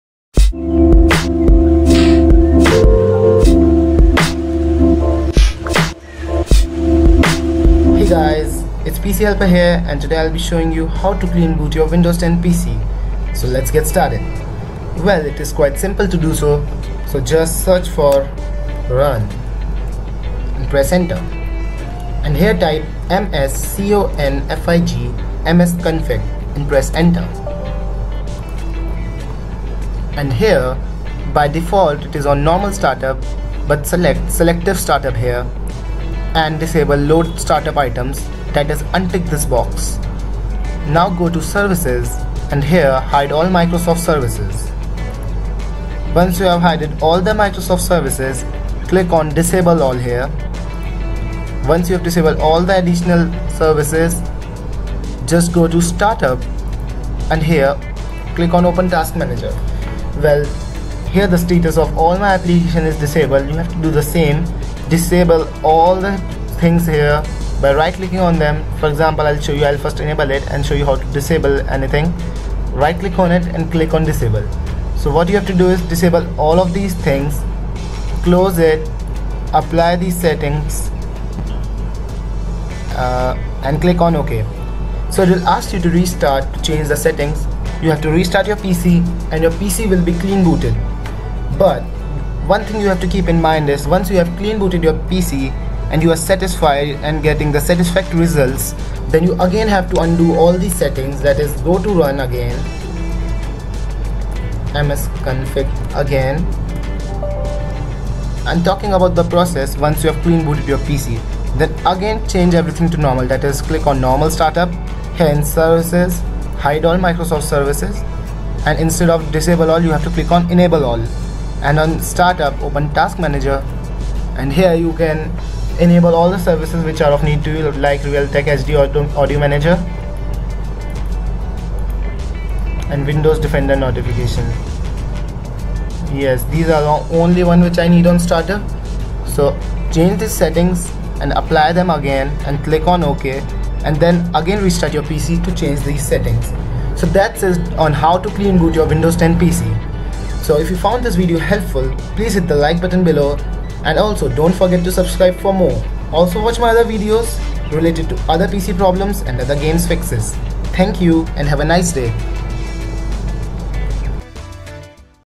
How To Clean Boot Windows 10 PC | QUICK GUIDE JUNE 2018
In this video I've shown how to clean boot Windows 10 PC in about 2 minutes.
Note:- Once You've clean booted your PC then please do remember to revert all the settings to default again.